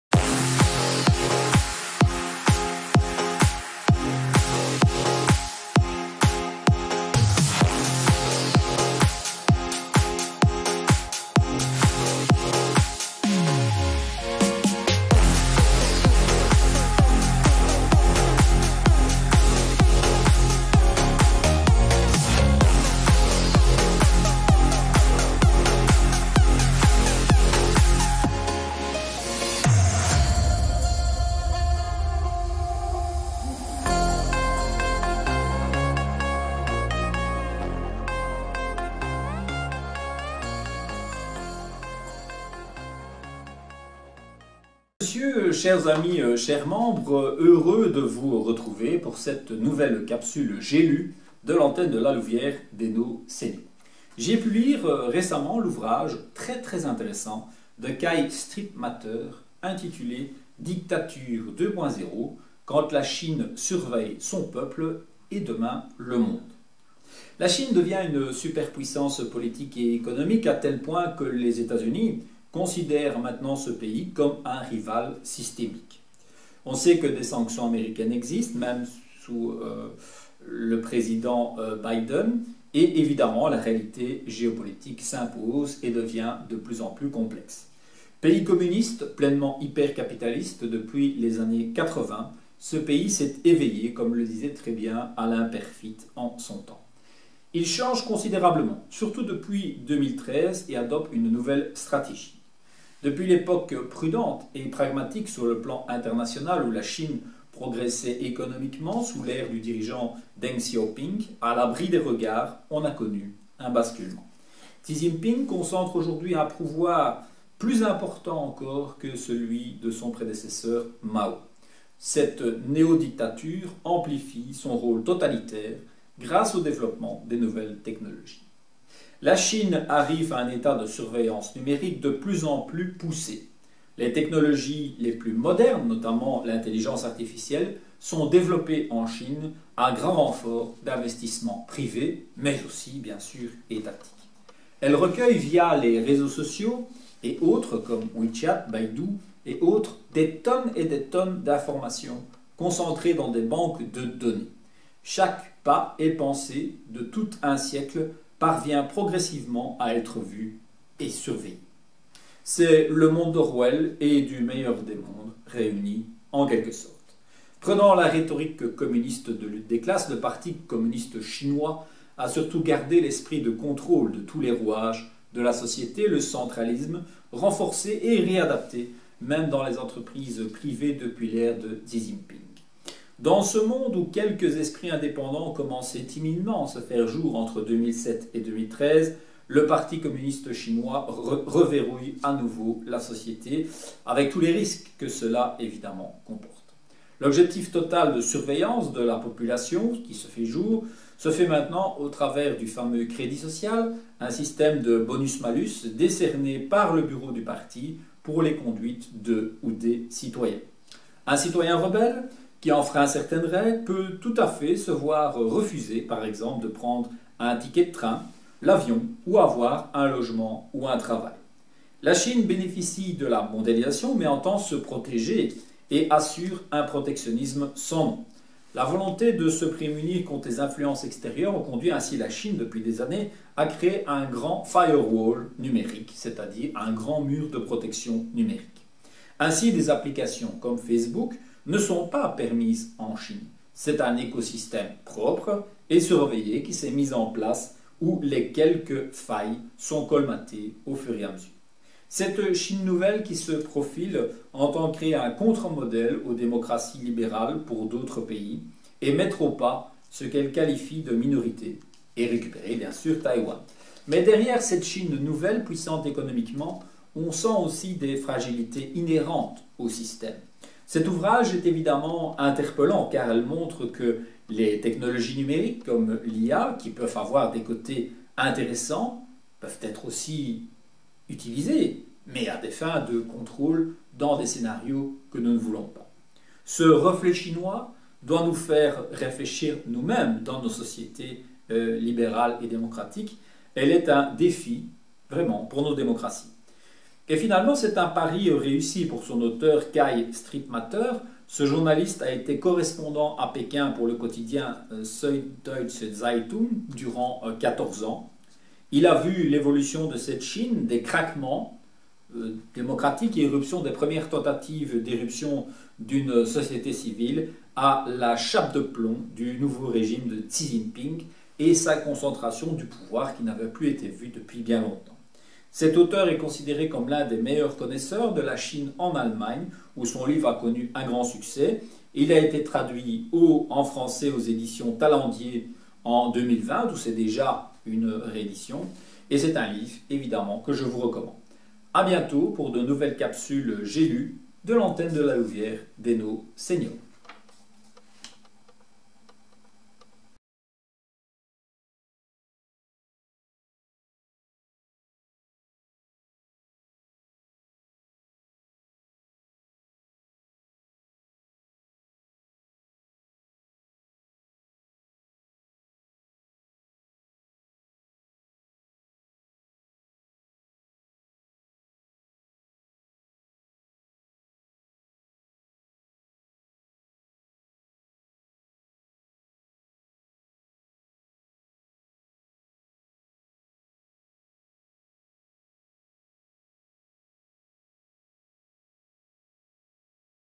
Capsule "J'ai lu" portant sur le livre Dictature 2 0
Nous vous recommandons la lecture de cet ouvrage fort intéressant donné par un journaliste spécialisée de la Chine contemporaine ...